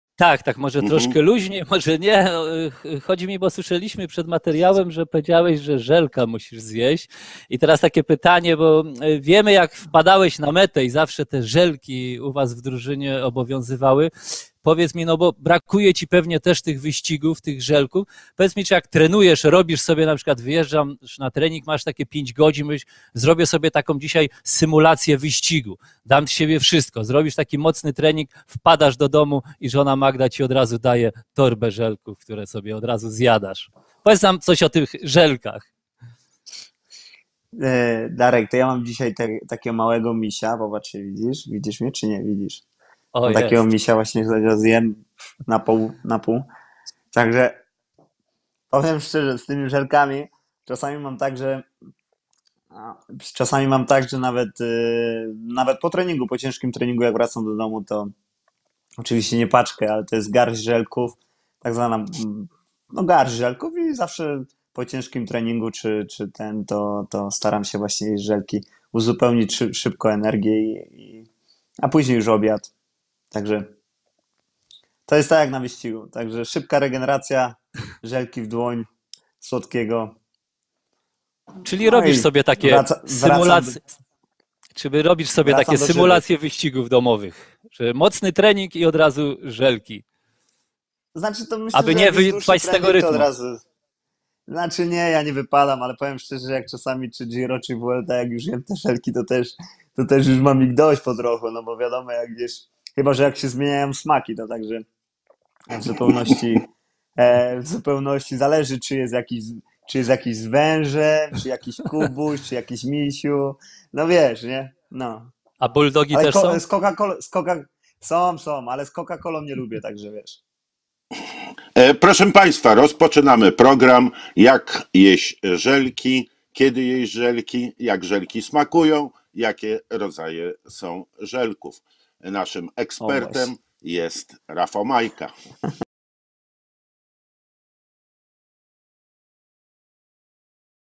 Dlaczego RAFAŁ MAJKA lubi jeść ŻELKI po wyścigu lub ciężkim treningu?
Dlaczego Rafał Majka lubi jeść żelki po wyścigu lub ciężkim treningu? Chętniej zjada misie czy węże? Za jakim smakiem nie przepada?
Sprawdźcie odpowiedzi na te i inne pytania!